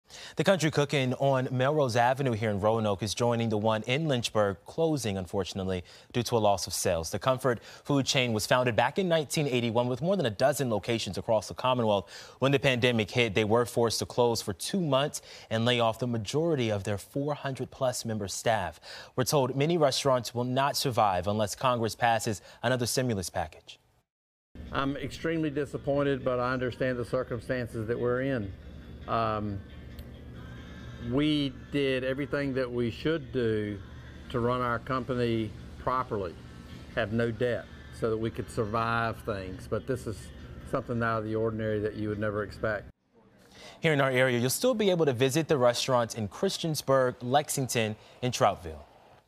Country Cookin' in Roanoke closes for good
Roanoke's Country Cookin' is closing due to loss of sales,